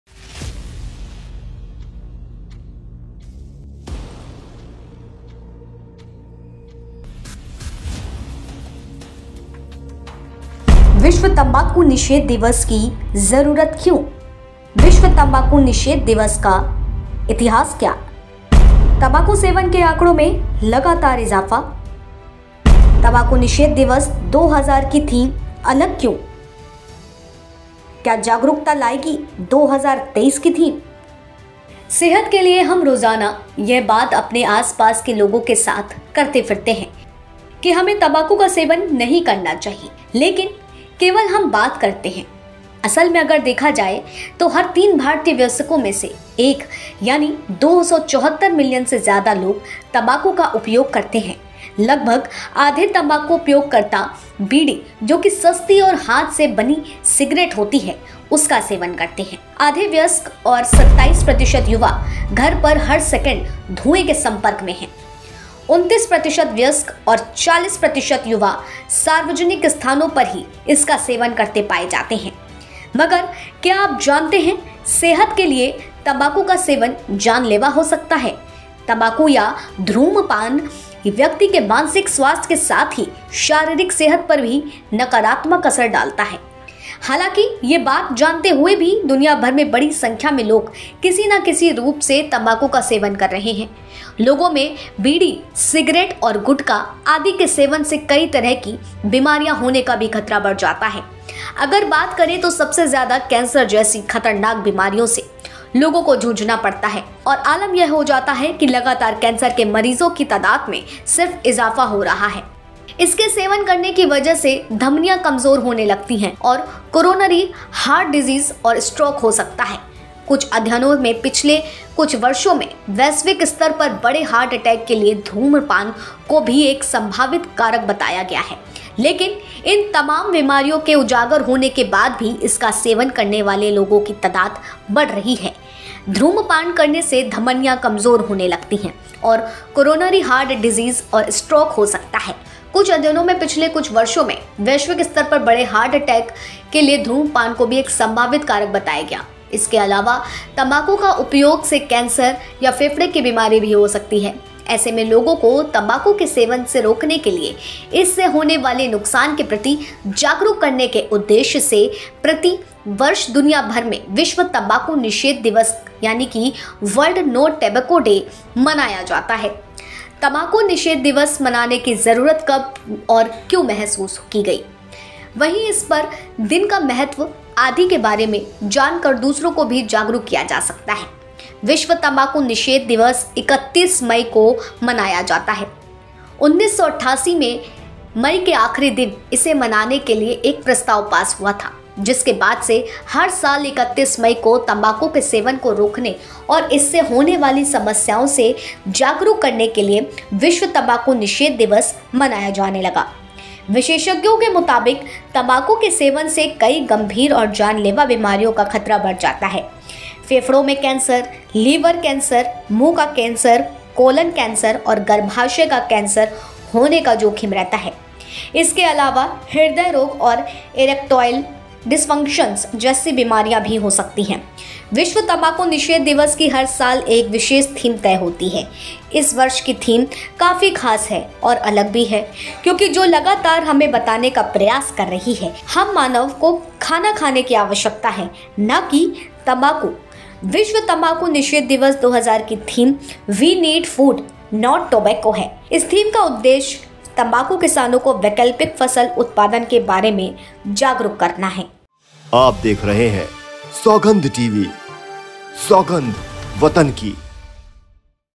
आज मनाया जा रहा है विश्व तंबाकू निषेध दिवस, जानें World No Tobacco Day की थीम, इतिहास और महत्व।
World No Tobacco Day is being celebrated today, know the theme, history and importance of World No Tobacco Day.